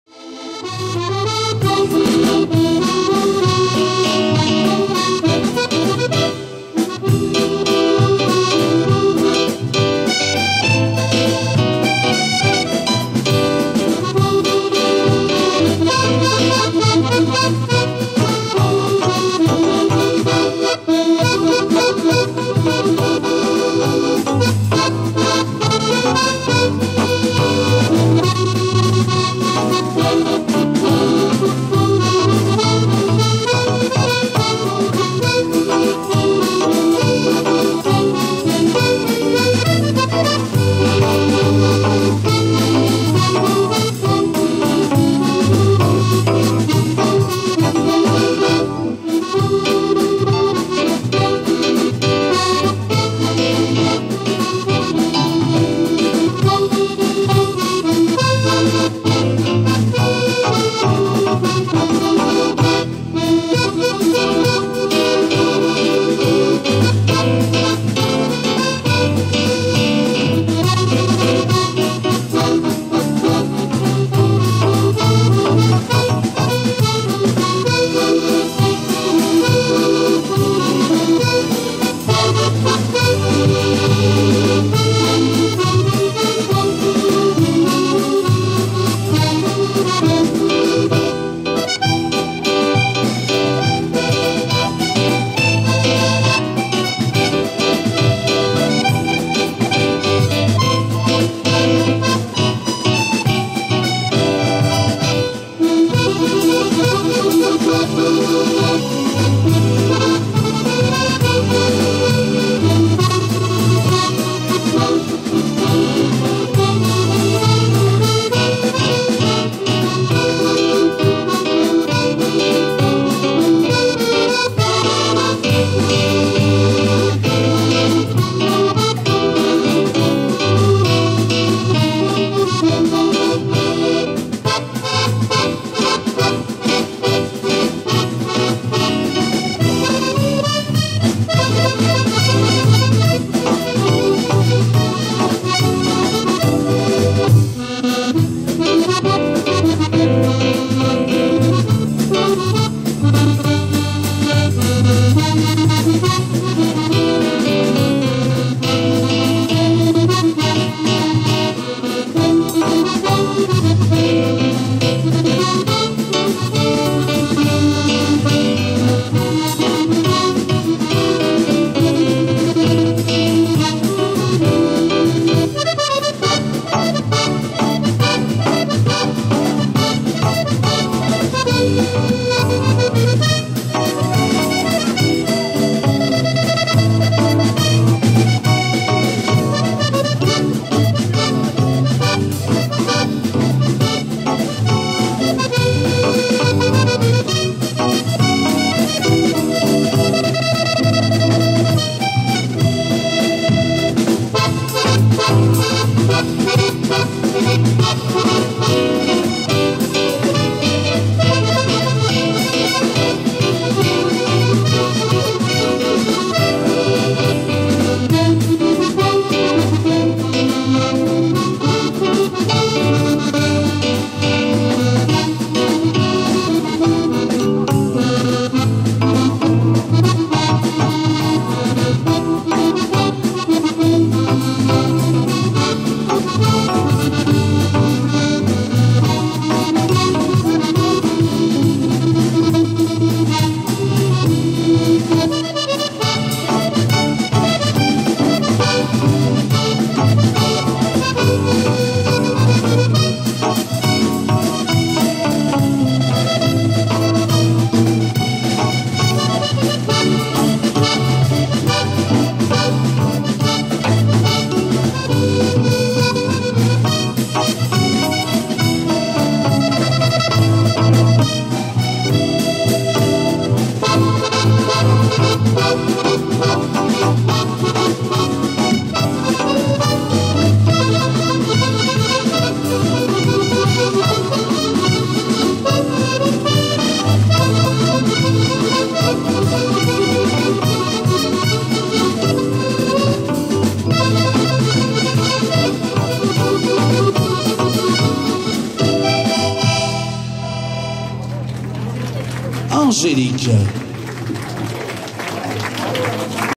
Angélique NEUVILLE Au bon temps Aurillac oct 2018 Valses
Gala d'accordéon Au bon temps à Aurillac (15) chez Gérard et Christiane Lacaze - Vidéo et réalisation Alexandra Astorg (Sandy)